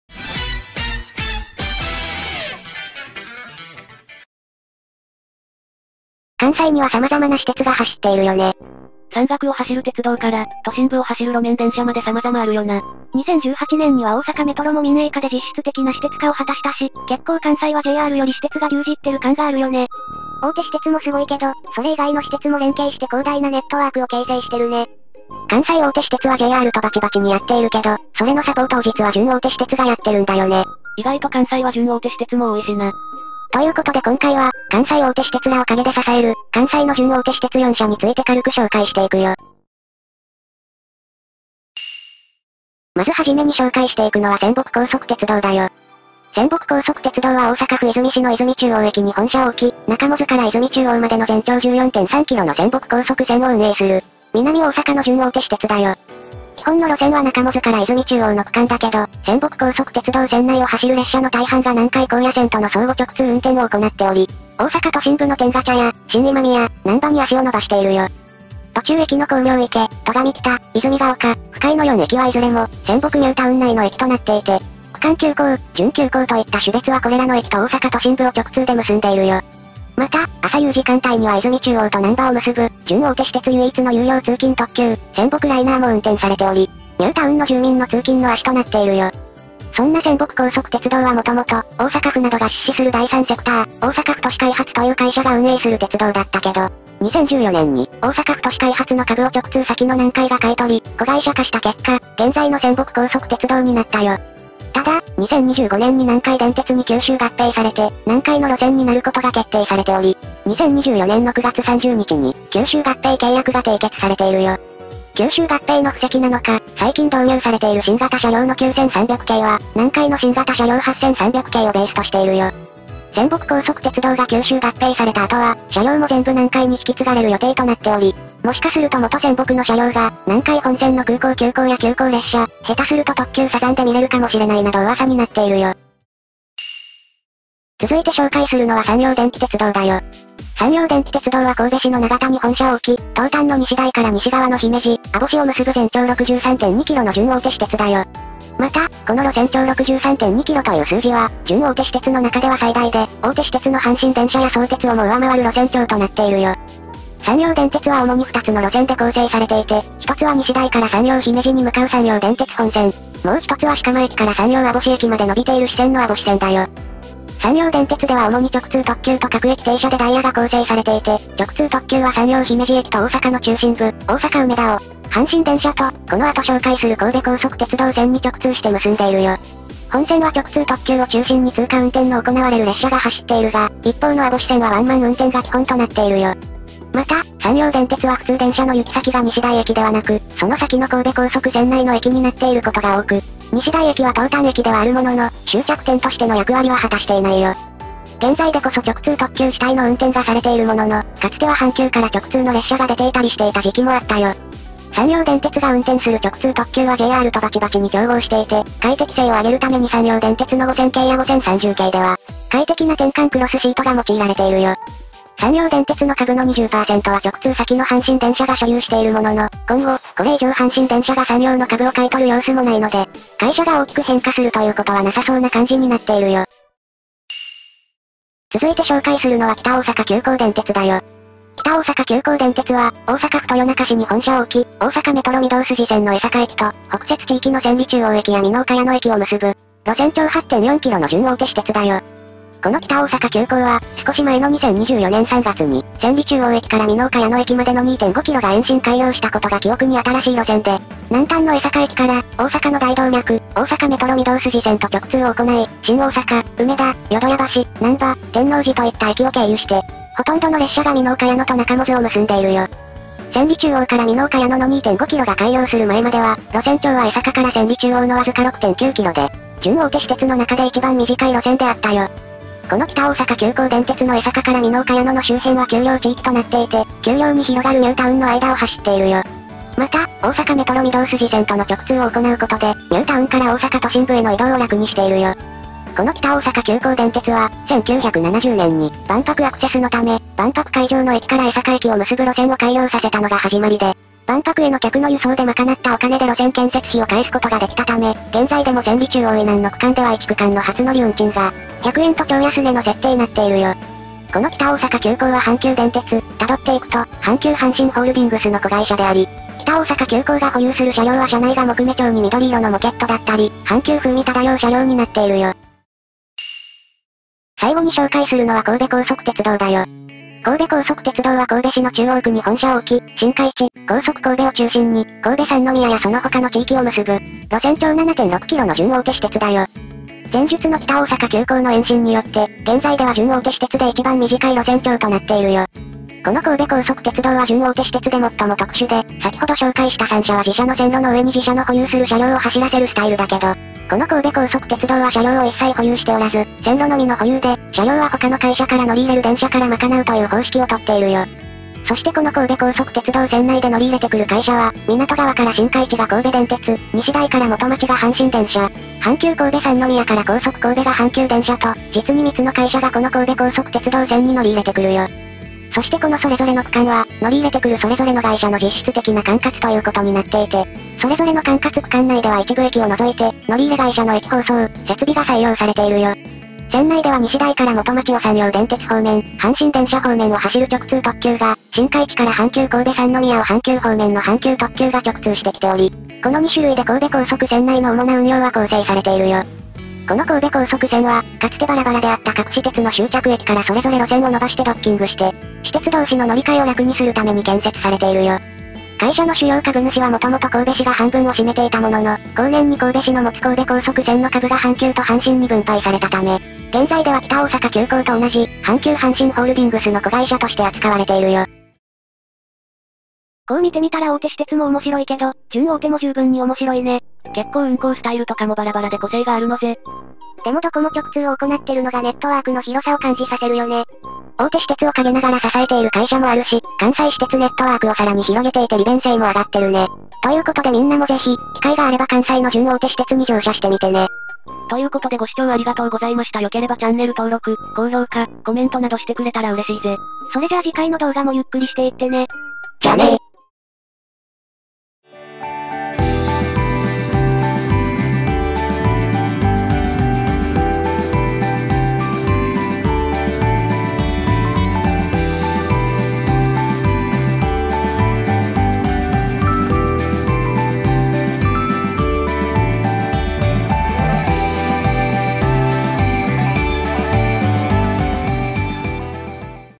【名・迷列車で行こう】個性がそれぞれ強い！？関西に存在する4つの「準大手私鉄」を紹介していく！【ゆっくり解説】【山陽電鉄】【北大阪急行】【泉北高速鉄道】【神戸高速鉄道】
投稿遅れた定期
※この動画は「東方project」に基づき作成した二次創作動画です。

・ゆっくり登場キャラ...霊夢・魔理沙・フラン・チルノ

YMM4